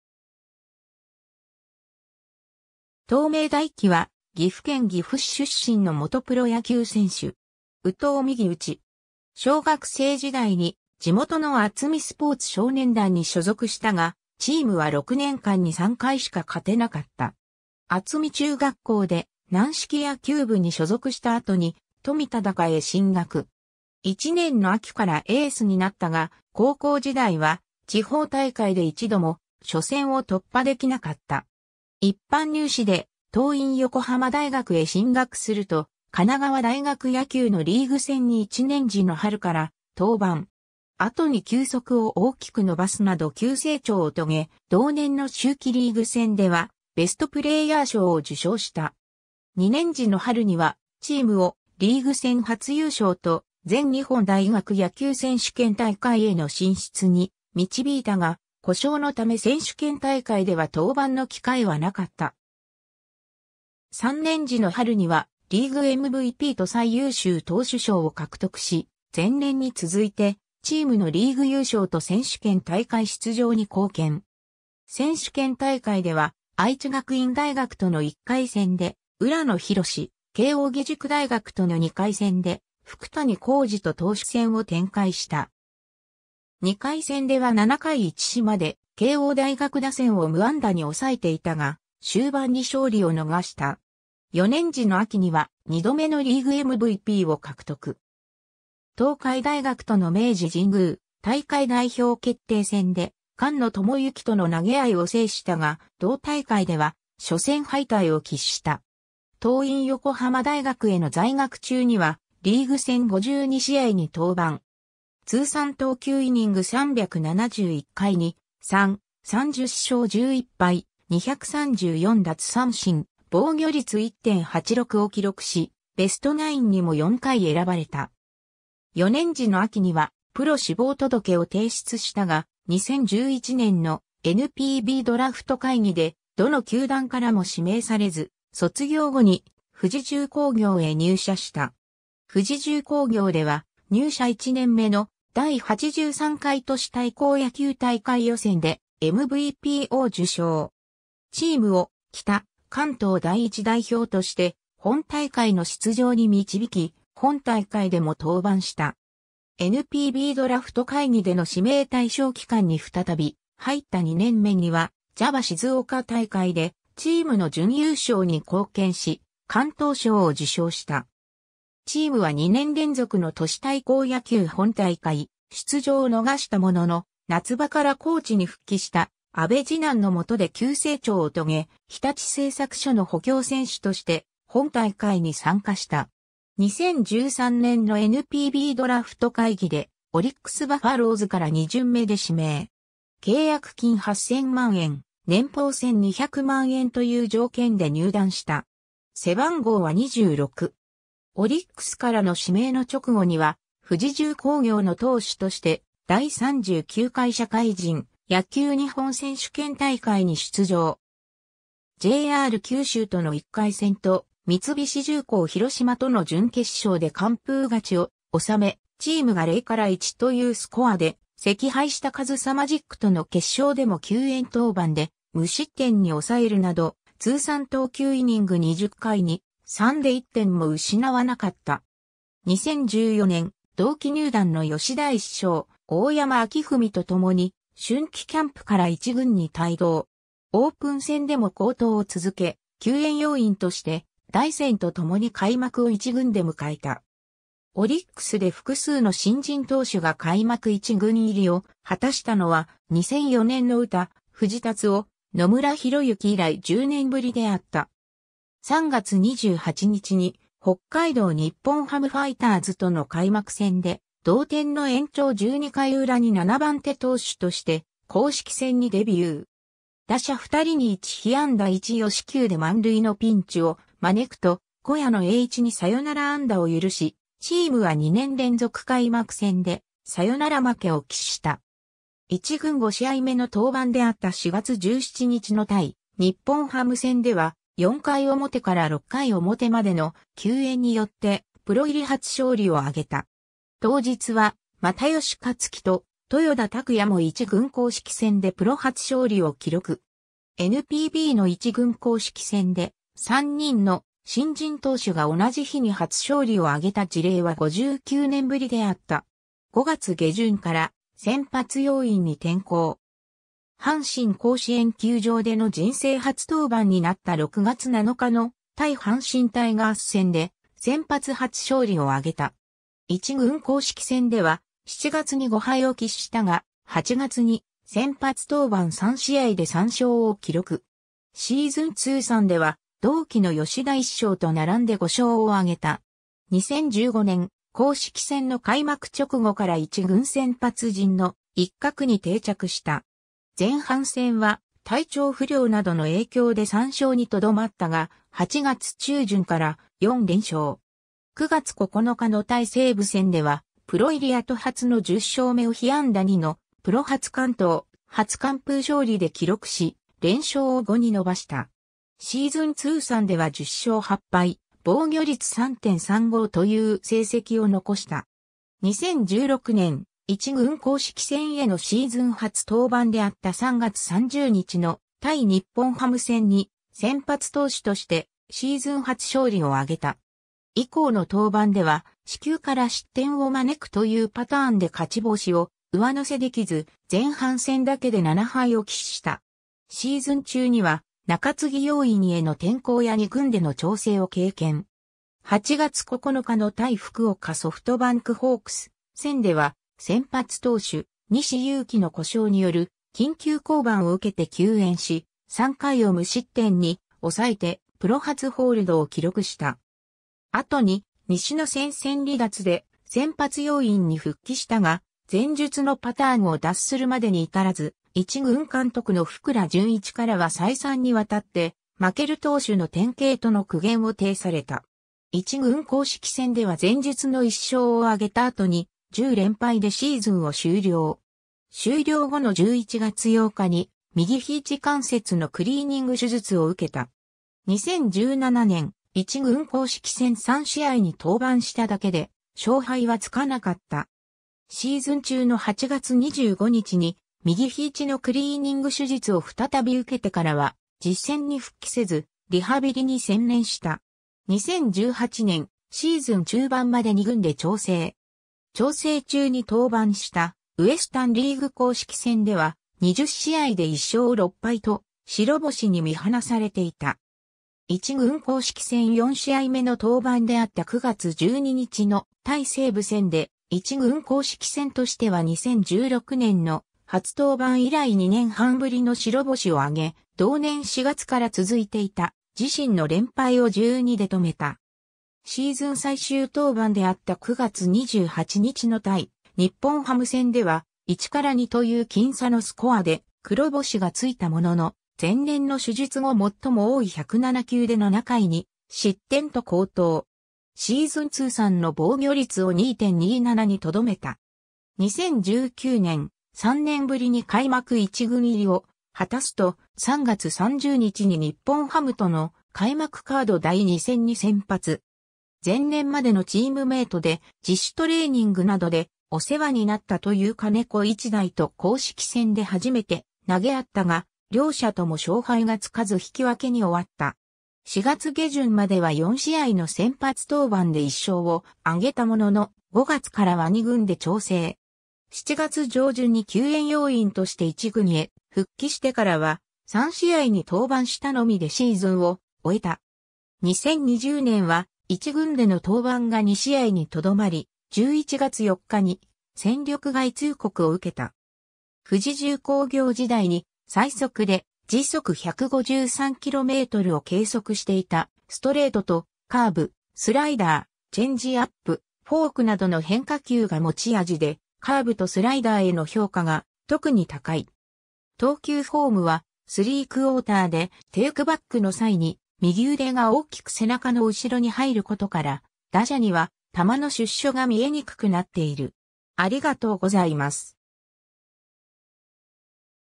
東明大貴
東明 大貴（とうめい だいき、1989年6月15日 - ）は、岐阜県岐阜市出身の元プロ野球選手（投手）。
右投右打。
 小学生時代に地元の厚見スポーツ少年団に所属したが、チームは6年間に3回しか勝てなかった。
厚見中学校で軟式野球部に所属した後に、富田高へ進学。
1年の秋からエースになったが、高校時代は地方大会で一度も初戦を突破できなかった。
 一般入試で桐蔭横浜大学へ進学すると、神奈川大学野球のリーグ戦に1年時の春から登板。
後に球速を大きく伸ばすなど急成長を遂げ、同年の秋季リーグ戦では、ベストプレーヤー賞を受賞した。
2年時の春には、チームをリーグ戦初優勝と全日本大学野球選手権大会への進出に導いたが、故障のため選手権大会では登板の機会はなかった。
3年時の春には、リーグMVPと最優秀投手賞を獲得し、前年に続いてチームのリーグ優勝と選手権大会出場に貢献。
選手権大会では、愛知学院大学との1回戦で浦野博司、慶應義塾大学との2回戦で福谷浩司と投手戦を展開した。
2回戦では7回1死まで慶応大学打線を無安打に抑えていたが、終盤に勝利を逃した。
4年時の秋には、2度目のリーグMVPを獲得。
東海大学との明治神宮大会代表決定戦で菅野智之との投げ合いを制したが、同大会では初戦敗退を喫した。
 桐蔭横浜大学への在学中には、リーグ戦52試合に登板。
通算投球イニング371回2/3、30勝11敗、234奪三振、防御率1.86を記録し、ベストナインにも4回選ばれた。
4年時の秋にはプロ志望届を提出したが、2011年のNPBドラフト会議でどの球団からも指名されず、卒業後に富士重工業へ入社した。
 富士重工業では、入社1年目の第83回都市対抗野球大会予選でMVPを受賞。
チームを北関東第1代表として本大会の出場に導き、本大会でも登板した。
NPBドラフト会議での指名対象期間に再び入った2年目には、JABA静岡大会でチームの準優勝に貢献し、敢闘賞を受賞した。
チームは2年連続の都市対抗野球本大会出場を逃したものの、夏場からコーチに復帰した阿部次男の下で急成長を遂げ、日立製作所の補強選手として本大会に参加した。
2013年のNPBドラフト会議で、オリックス・バファローズから2巡目で指名。
契約金8,000万円、年俸1,200万円（金額は推定）という条件で入団した。
 オリックスからの指名の直後には、富士重工業の投手として第39回社会人野球日本選手権大会に出場。
JR九州との1回戦と三菱重工広島との準決勝で完封勝ちを収め、チームが0 - 1というスコアで惜敗したかずさマジックとの決勝でも救援登板で無失点に抑えるなど、通算投球イニング20回2/3で1点も失わなかった。
 2014年、同期入団の吉田一将・大山暁史とともに、春季キャンプから一軍に帯同。
オープン戦でも好投を続け、救援要員として、大山と共に開幕を一軍で迎えた。
オリックスで複数の新人投手が開幕一軍入りを果たしたのは、2004年の歌藤達夫・野村宏之以来10年ぶりであった。
3月28日に、北海道日本ハムファイターズとの開幕戦（札幌ドーム）で、同点の延長12回裏に7番手投手として公式戦にデビュー。
打者2人に1被安打1与四球で満塁のピンチを招くと、小谷野栄一にサヨナラ安打を許し、チームは2年連続開幕戦でサヨナラ負け（2リーグ分立後のNPB史上初の記録）を喫した。
一軍5試合目の登板であった4月17日の対日本ハム戦（ほっともっとフィールド神戸）では、4回表から6回表までの救援によって、プロ入り初勝利を挙げた。
当日は、又吉克樹（中日ドラゴンズ）と豊田拓矢（埼玉西武ライオンズ）も一軍公式戦でプロ初勝利を記録。
NPBの一軍公式戦で、3人の新人投手が同じ日に初勝利を挙げた事例は59年ぶりであった。
5月下旬から先発要員に転向。
阪神甲子園球場での人生初登板になった6月7日の対阪神タイガース戦で、先発初勝利を挙げた。
一軍公式戦では、7月に5敗を喫したが、8月に先発登板3試合で3勝を記録。
シーズン通算では、同期の吉田一将と並んで5勝を挙げた。
 2015年、公式戦の開幕直後から一軍先発陣の一角に定着した。
前半戦は体調不良などの影響で3勝にとどまったが、8月中旬から4連勝。
9月9日の対西武戦（西武プリンスドーム）では、プロ入り後初の10勝目を...